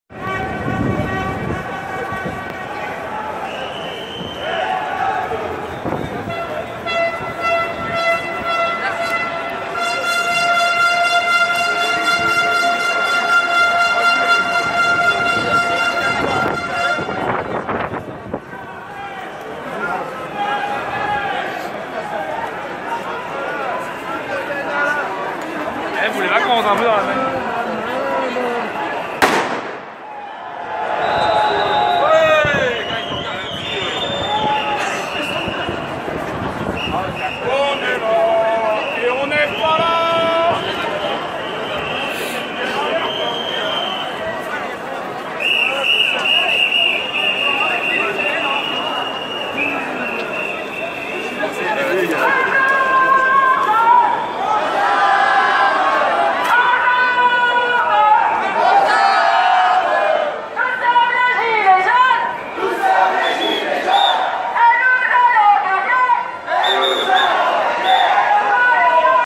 ♦️فیلمی از تظاهرات جلیقه زرد های فرانسه امروز شنبه ۲۵ اسفند ۹۷ در پاریس
ارسالی یکی از هموطنان به کانال بگونه